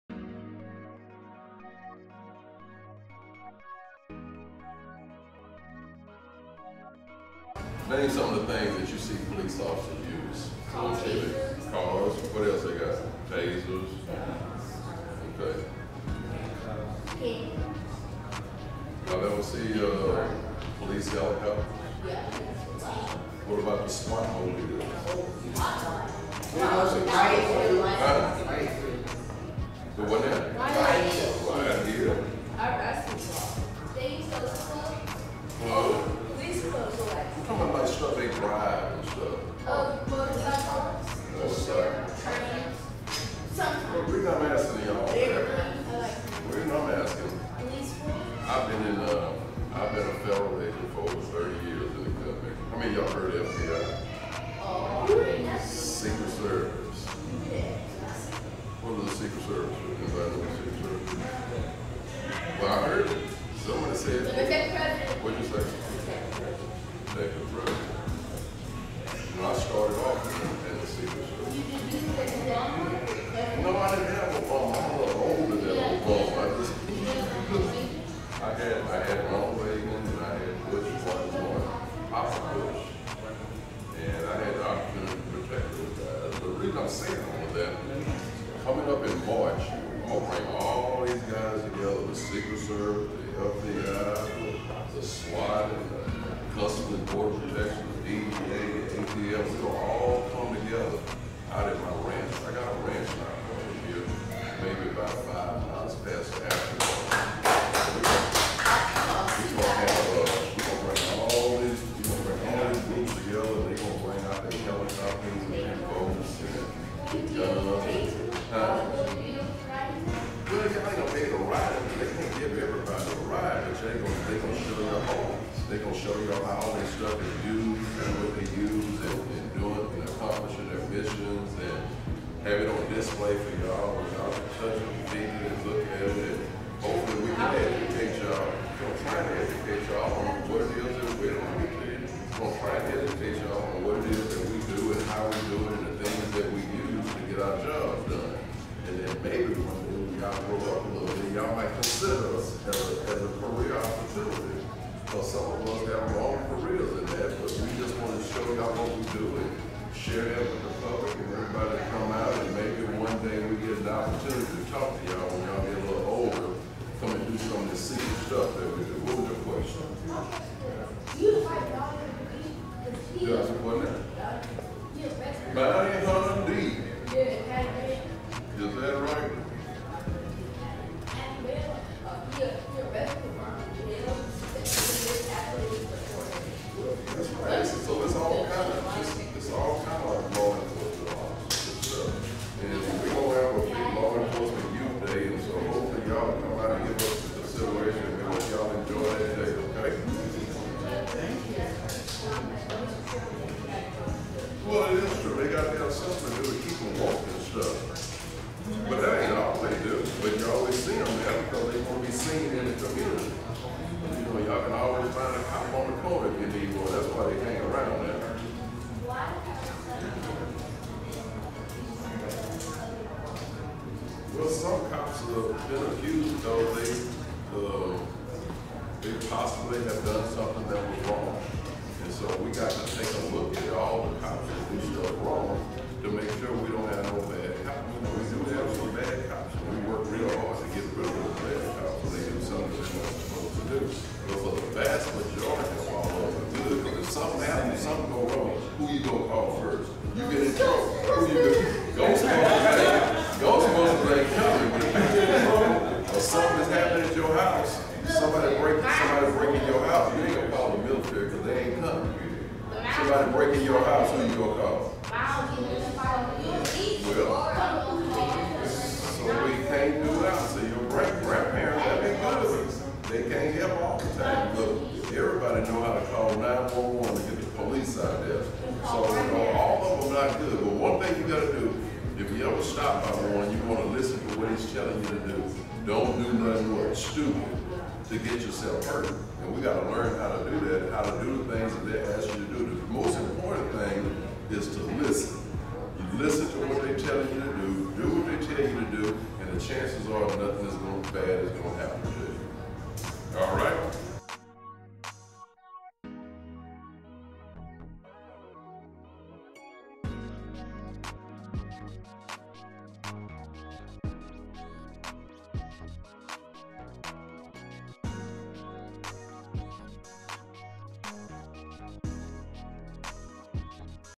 A Law Enforcement Message for Youth
A message given to The STEM Foundation students about Law Enforcement careers, as well as, what to do when confronted.